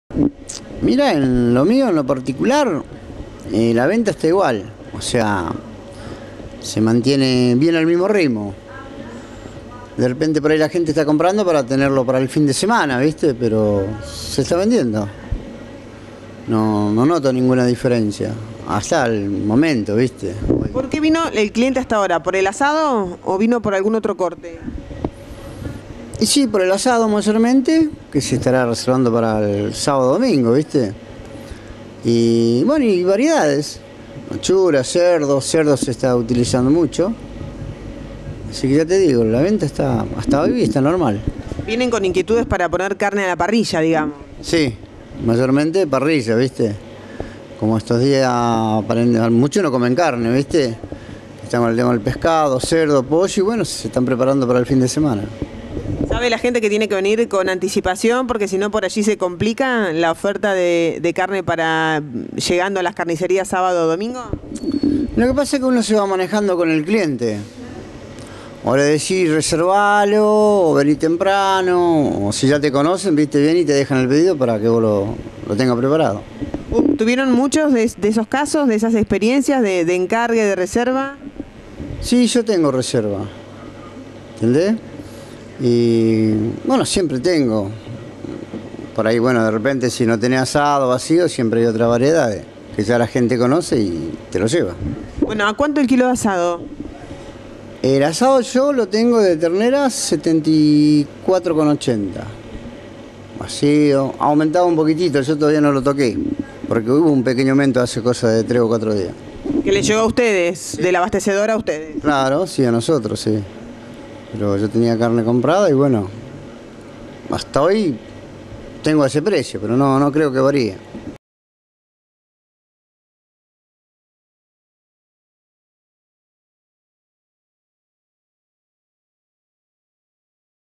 AUMENTO EL PRECIO DE LA CARNE
LA CARNE AUMENTO SU PRECIO PARA ESTAS PASCUAS, PERO A PESAR DE ELLO, MUCHOS PLATENSES CONTINUAN COMPRANDO Y GUARDANDO PARA EL FIN DE SEMANA.